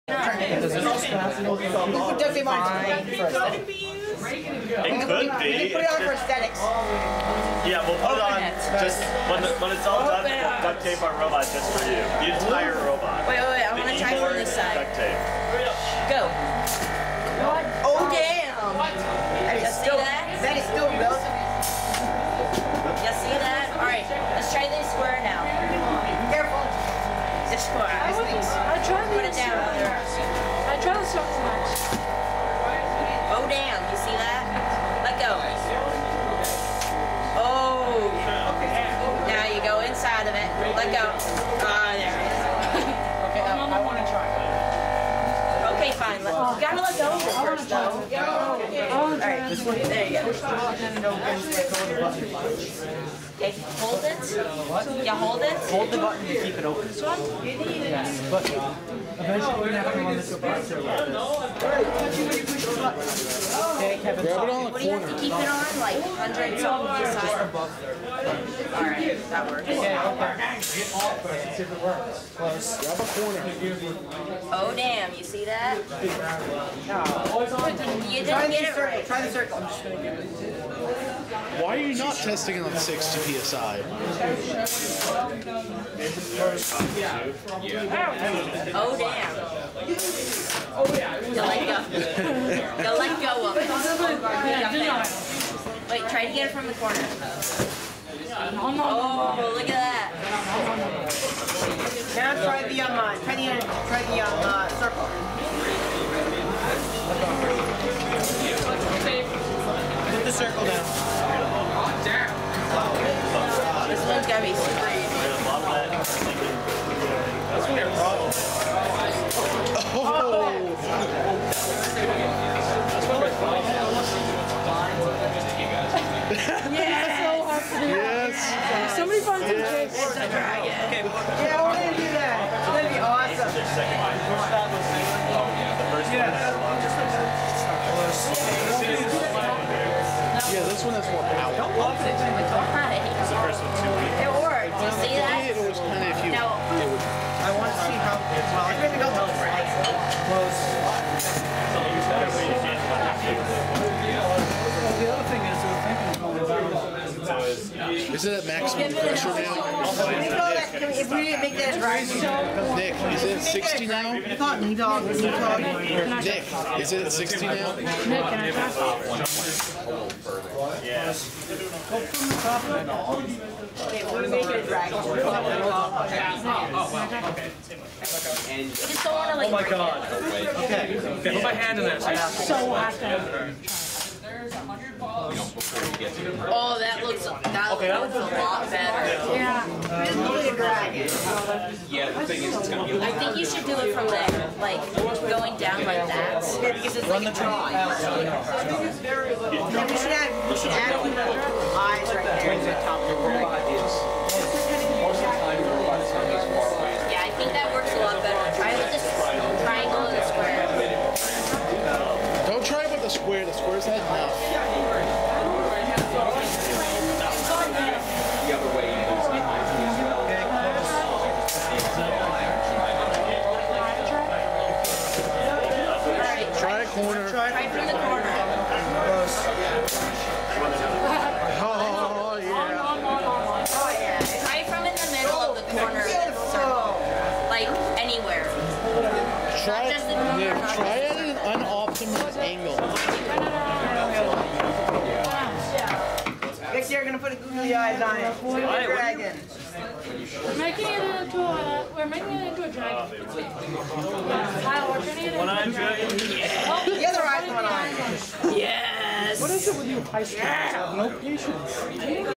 Chop Shop 166 - Prototyping our Grabber
We are testing a prototype for a grabber that seems to work for all directions for all shapes.